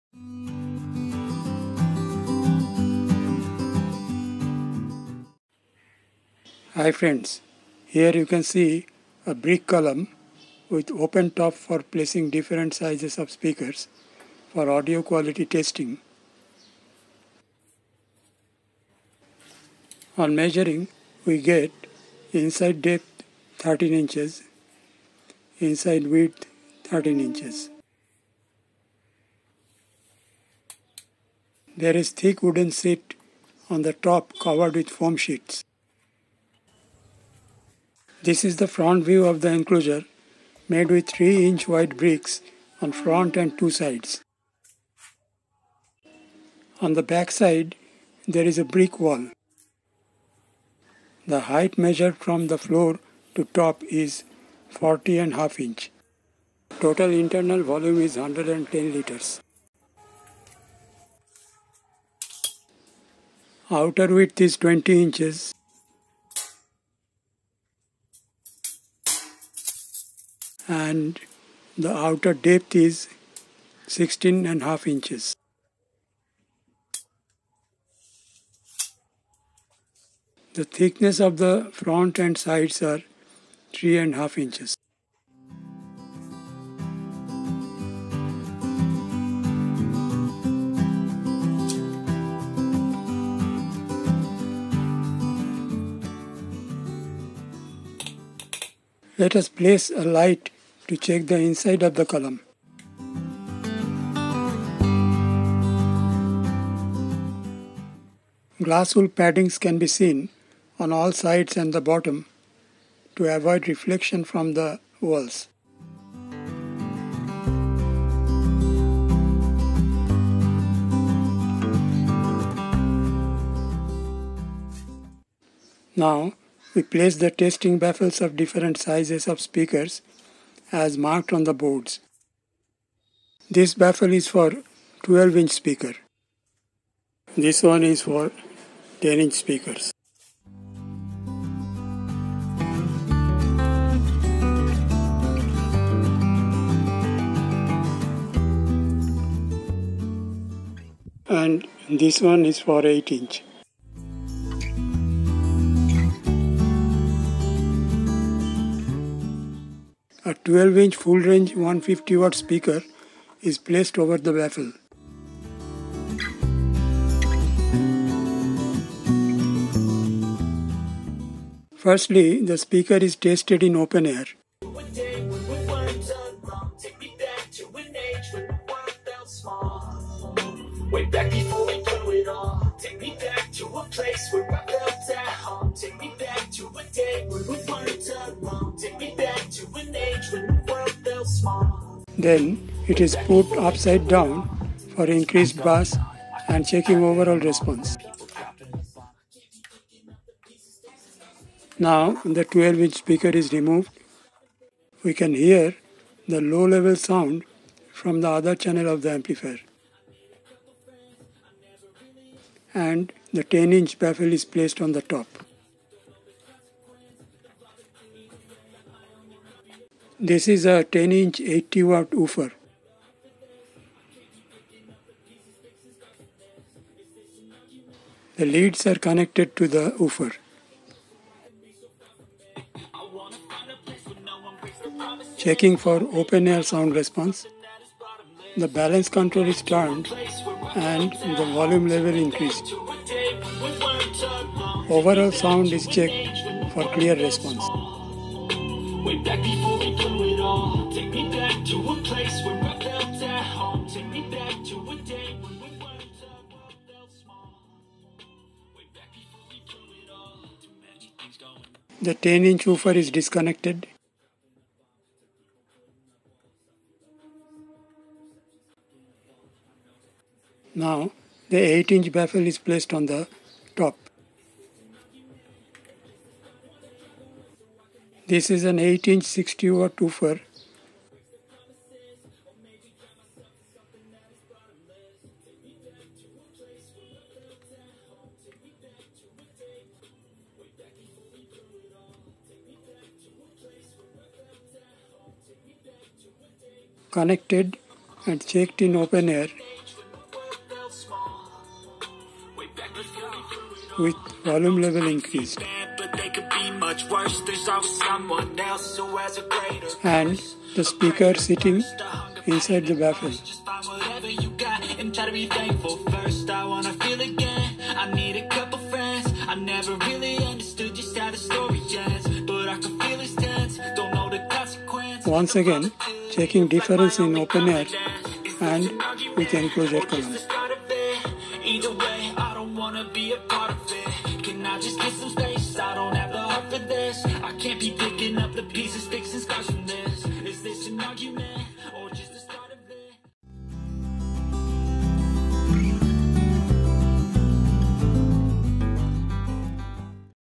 Testing Loudspeaker in Brick enclosure
When we check performance of a loudspeaker, the vibration of enclosure material make some sort of coloration in the overall response.
To avoid such problem, brick enclosure may be a good approach. Because of much higher density than wooden panels, brick enclosures are always better.
Of course there are some practical difficulties like fixed position, larger listening area etc.  That’s why such enclosure or column is recommended for testing set ups.
Here I have shown a simple arrangement for checking audio quality of different sizes of speakers using same column with changeable baffles. Thank you